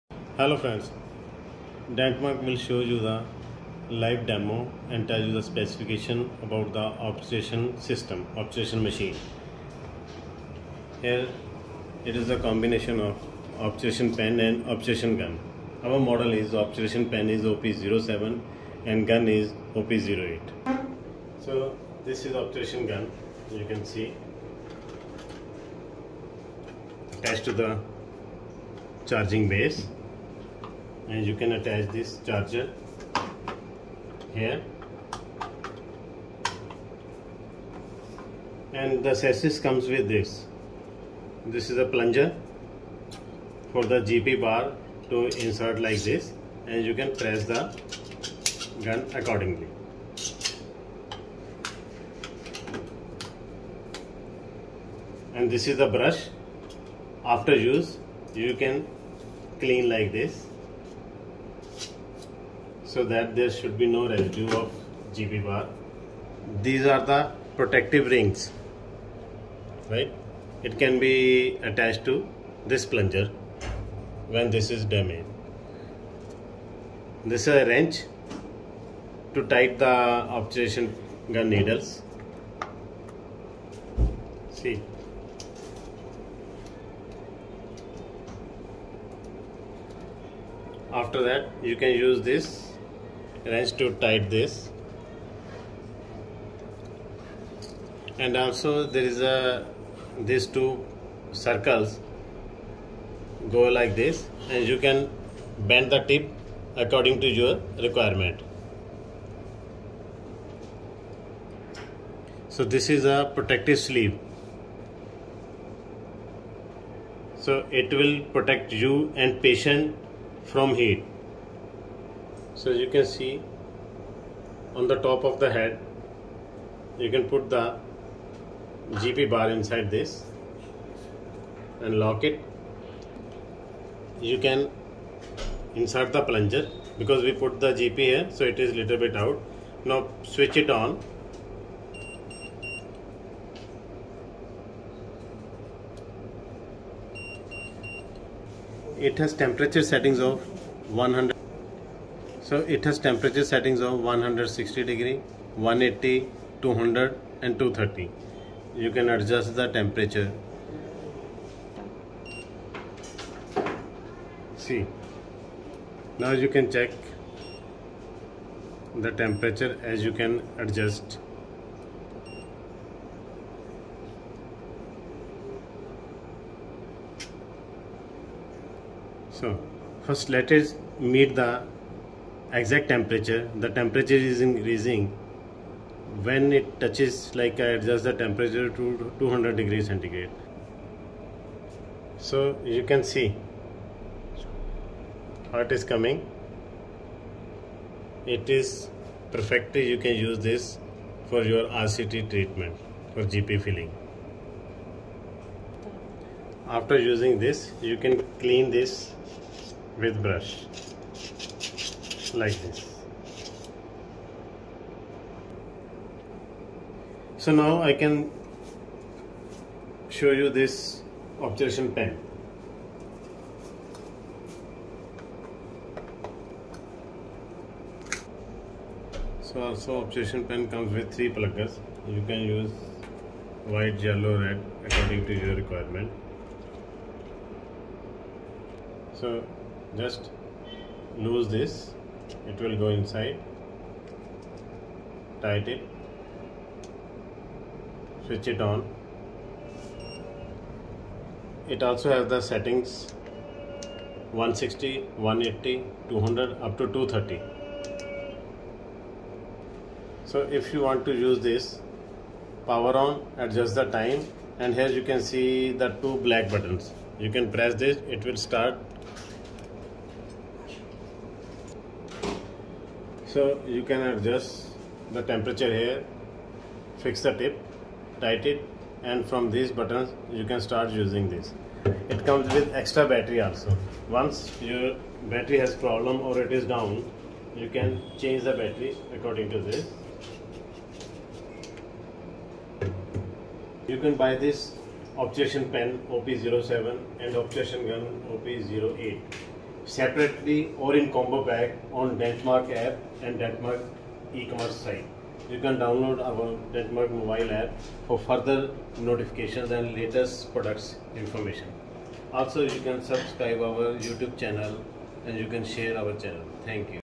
DENTMARK GUTTA PERCHA OBTURATION SYSTEM GUN+PEN 0P06 (HOW TO USE )
GP OBTURATION SYSTEM#OBTRA GUN PRICE#GP SYSTEM
A.Obturation Gun.

1. Wireless 3D filling design.

2. Comfortable holding and easy to operate.

3 .Accurate temperature control and thermal protector cap can prevent accidental scald.

4. Gun needle screw design can effectively prevent excessive glue.

5. It can be heated to 230â„ƒ in 15s, which can melt all kinds of gutta percha.

5. Large battery capacity can use longer time.

7. Auto idle power saving mode, the device will turn off automatically after 15 minutes of non-use.

B. Obturation Pen

1. Wireless design, easy to operate and light body.

2. Adjustable hot needle size and temperature mode, gutta perchavertical condensation technique can compress gutta percha more compact.

3. Own heating protection function, the heating maximum time of heater is 10s, more efficient and secure.

4. Heating key 360â„ƒ stereo operation, accurate top control , 3D filling more thoroughly.

5. The heater can be heated to  230â„ƒ in 5s.

6. Optional temperature:160/180/200/230â„ƒ